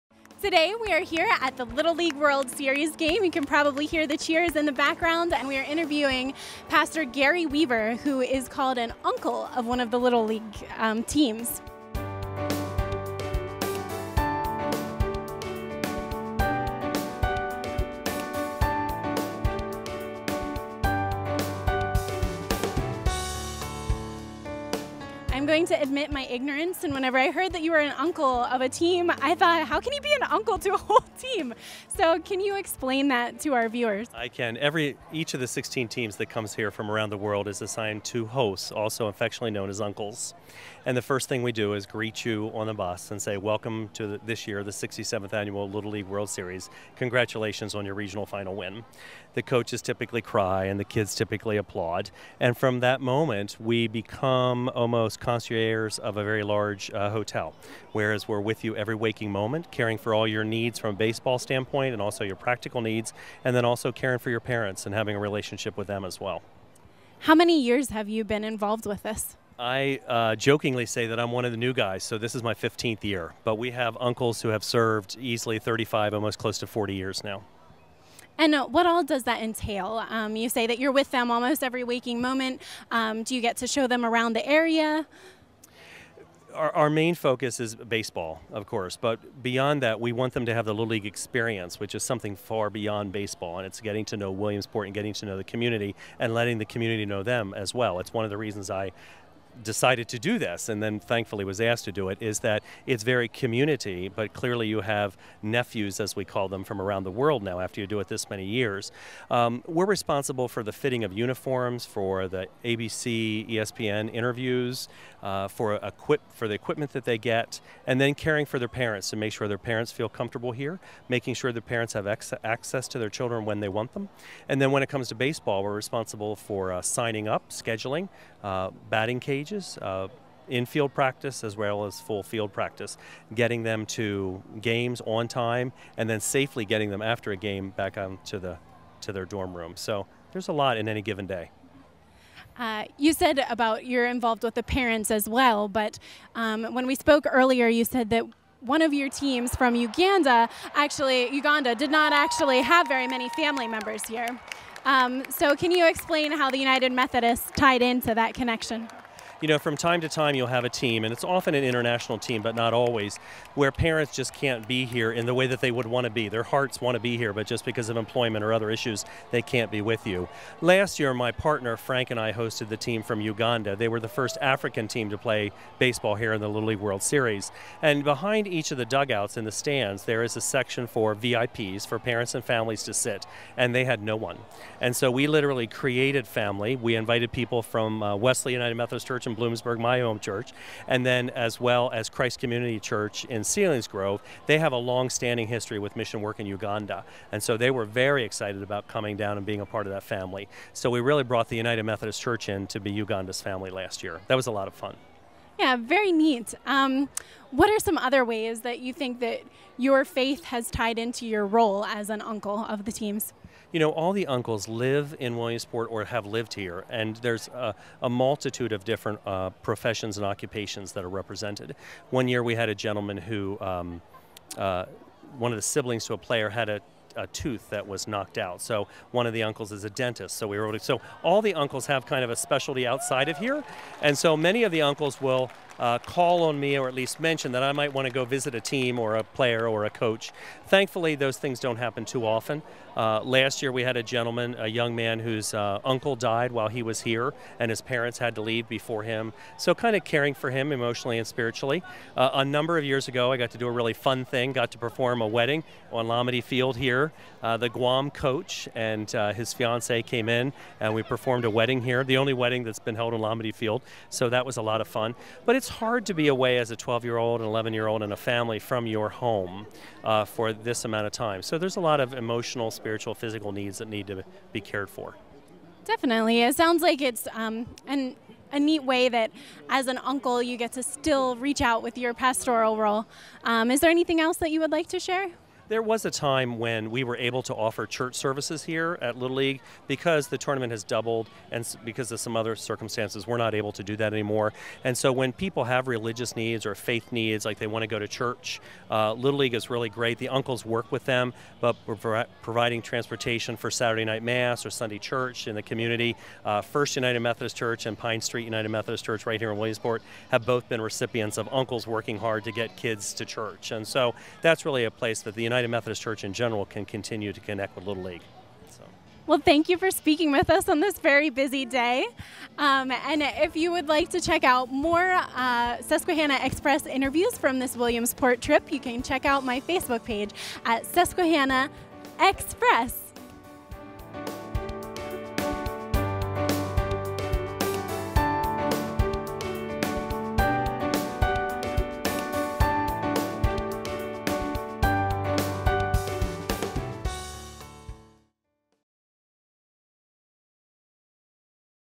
Susquehanna Xpress: Rev. Dr. Gary Weaver - Little League World Series
Brittany talks with Rev. Dr. Gary Weaver about what being an "uncle" means to a team in the Little League World Series.